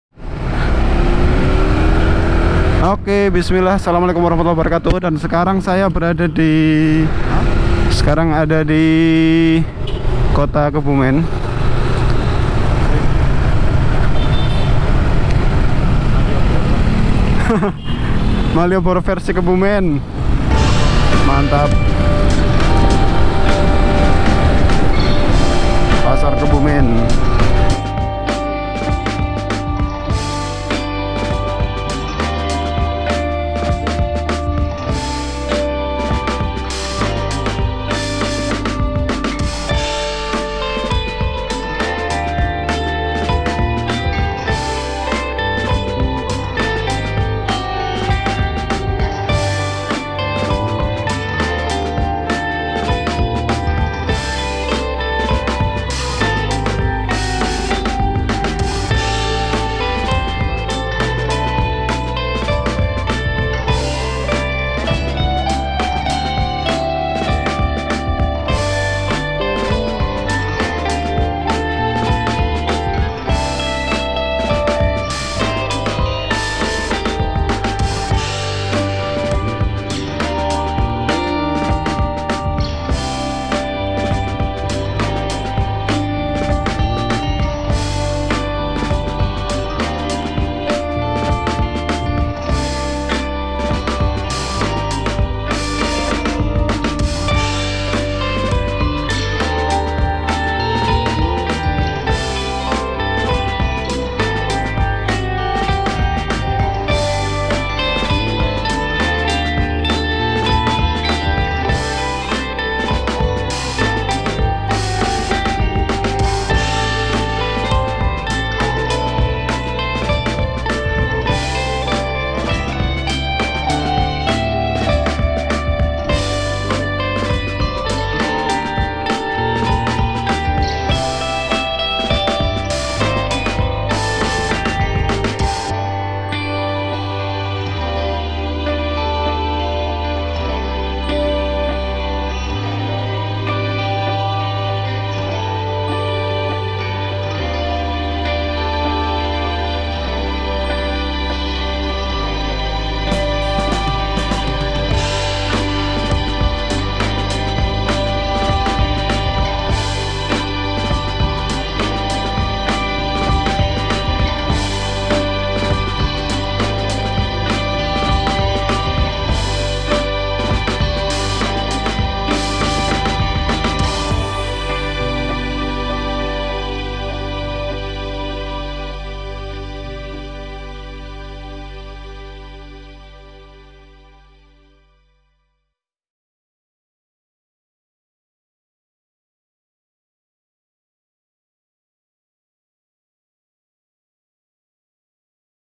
Yang Lagi Viral Di KEBUMEN!!!
Ternyata ada yang baru di kebumen nih. ngga sengaja dan ngga direncana.
Bagus juga pembangunannya, lebih rapi dan bersih, enak untuk nongkrong dan bagus juga untuk foto, sudah ada jalur sepedanya, mantap dah pokoknya. Lengkap dengan bangku tempat duduk dan lampu lampu di sepanjang jalan Soekarno Hatta. Lokasinya ada di Sebelah Timur Alun alun kota kebumen atau di sebelah pasar Tumenggungan kebumen.